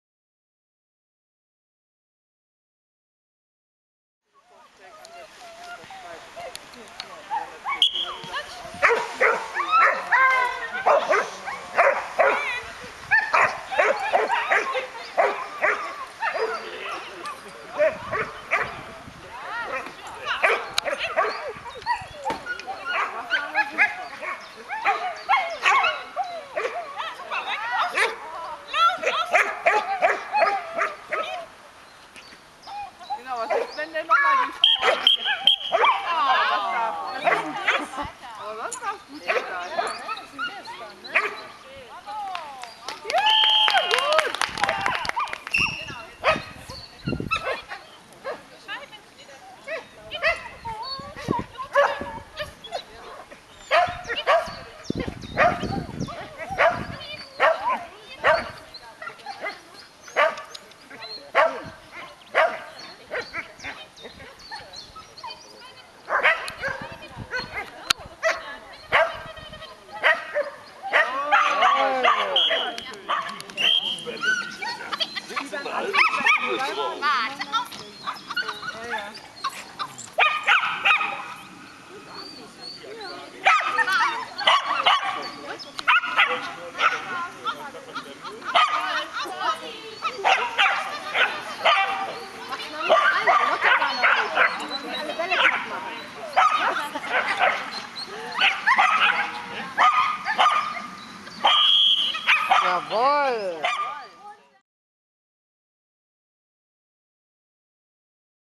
Treibball Spiel
Eins der momentan drei Treibball Spielen die im niederländischen Reglement vorkommen. Der Hund muß  den Ball eine vorgegebene Strecke durch nummerierte Durchänge treiben. Der Hundeführer darf sich überall im Spielfeld bewegen.